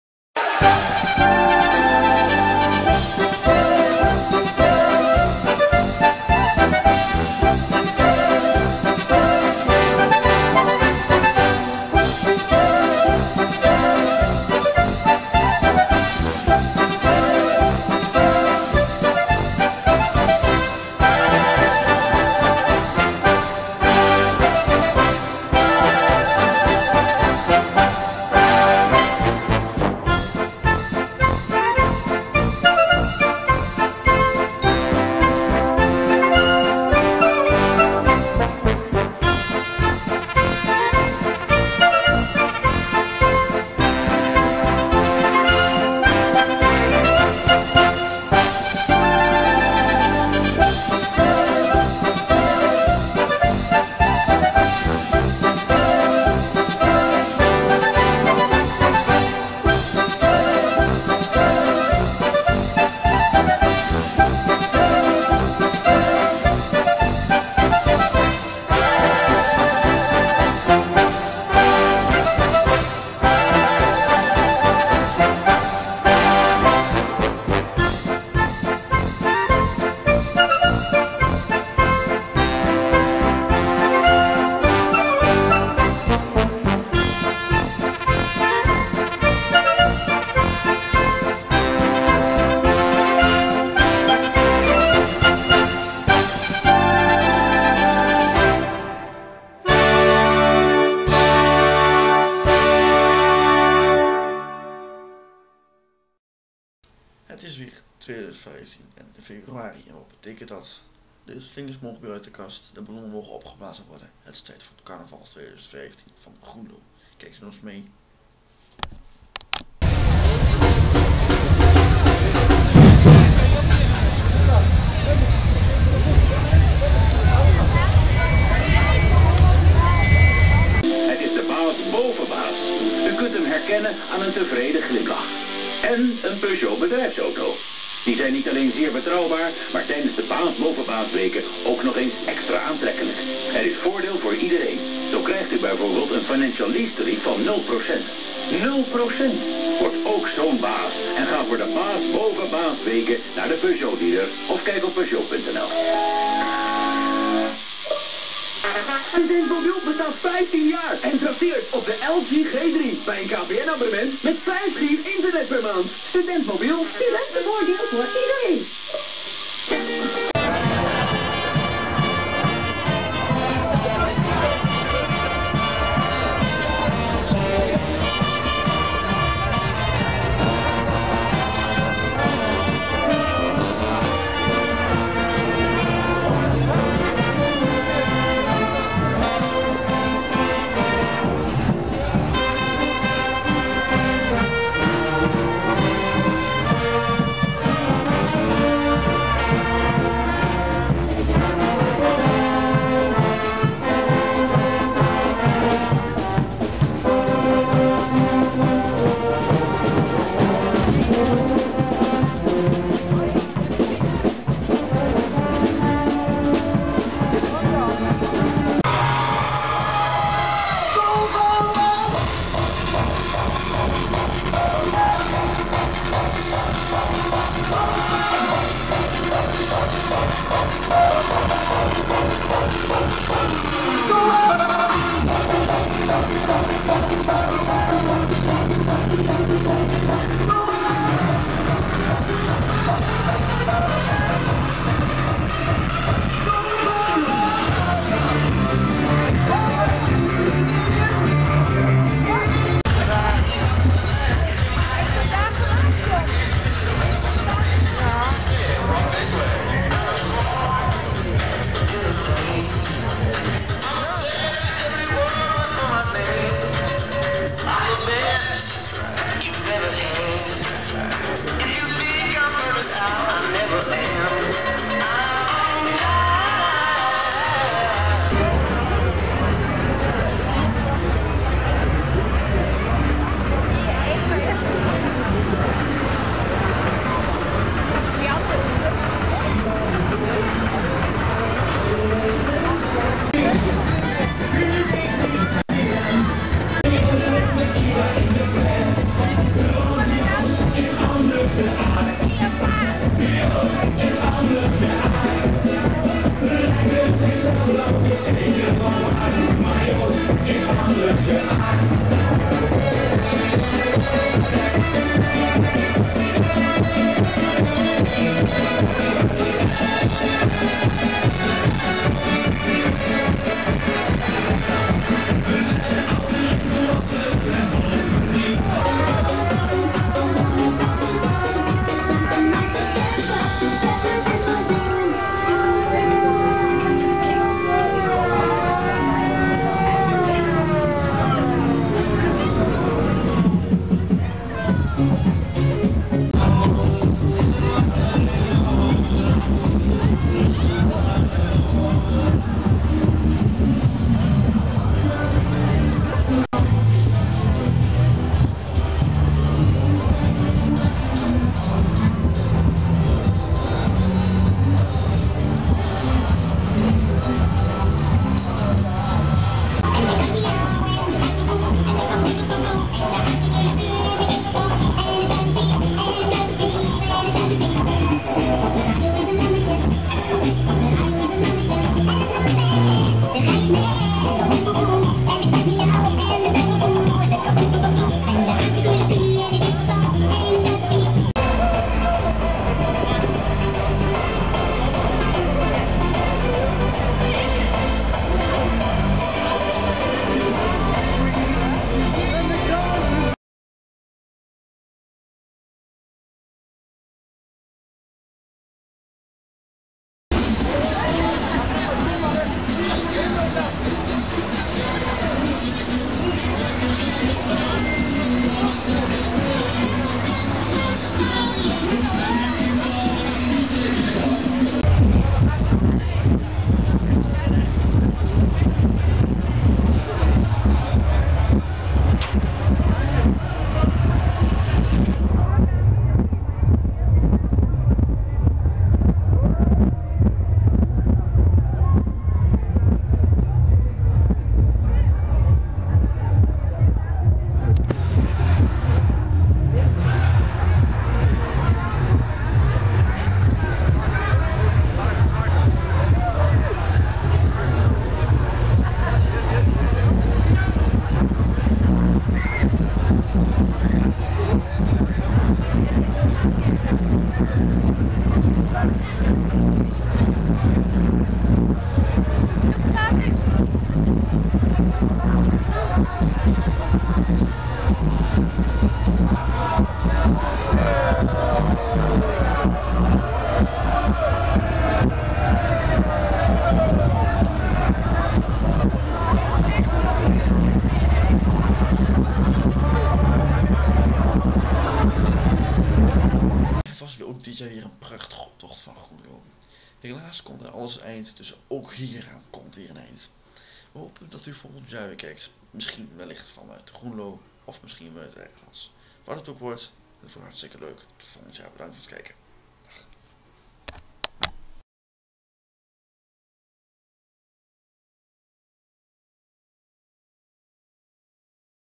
carnavalsoptocht 2015 groenlo TV Achterhoek
een jaarlijks compleet verslag van de carnavalsoptocht dit jaar vanuit Groenlo

met  camera: job de geus
presentator ; mitchell de graaf